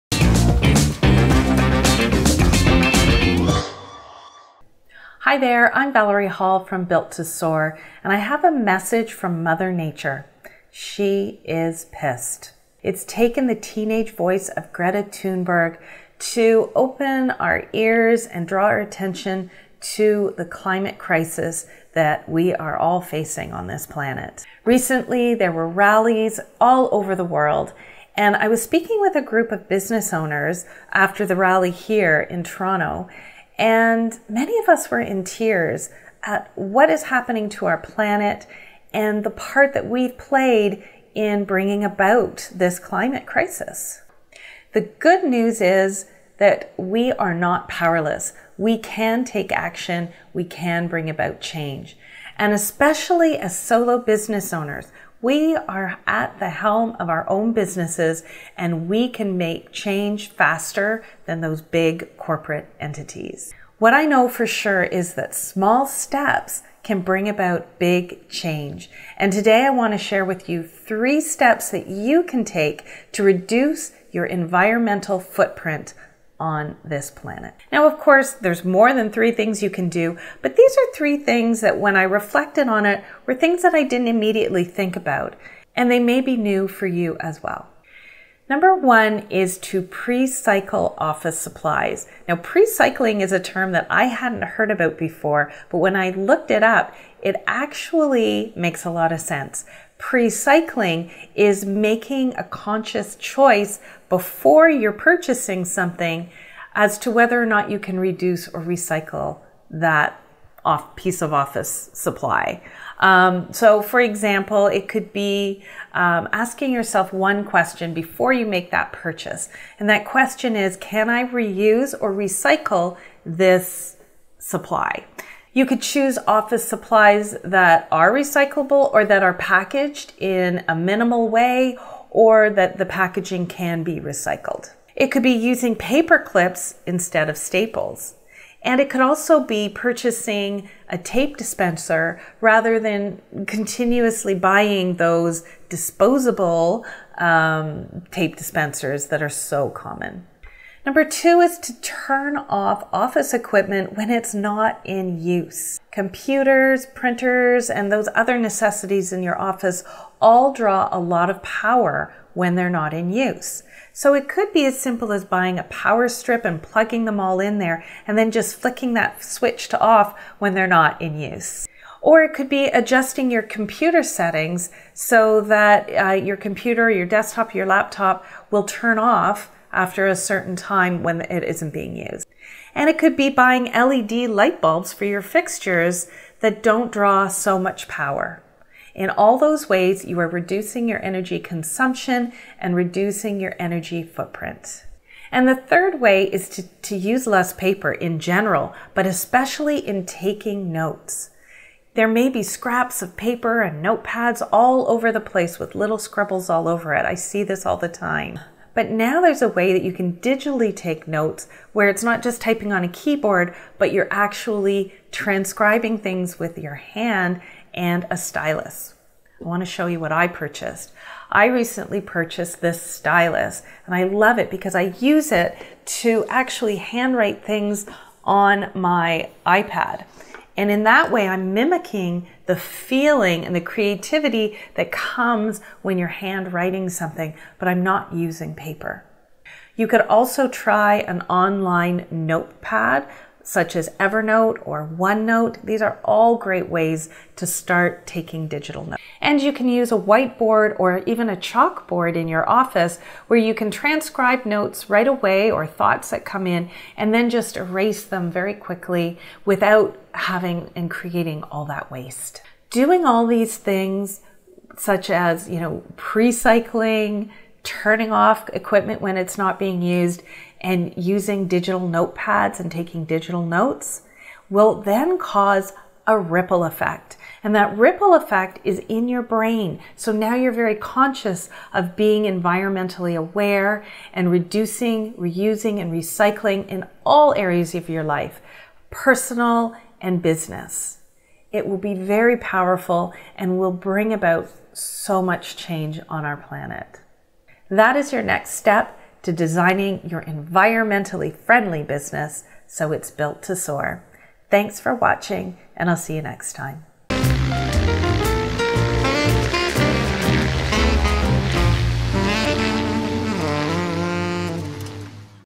3 ways your solo business can fight climate change
It has taken the teenage voice of Greta Thunberg to be her voice and cause people to pay attention to the environmental crisis that has been brewing for 30 years.

When it comes to the climate, it’s no longer business-as-usual

The truth is, all of us are guests on this planet, and we all have a responsibility to make change happen.

The good news is you’re not powerless in this fight.
Solo businesses can make a impact

As I reflected on what I could do in my own business, in this video I’ll share some things I’ll be doing to make my business environmentally friendly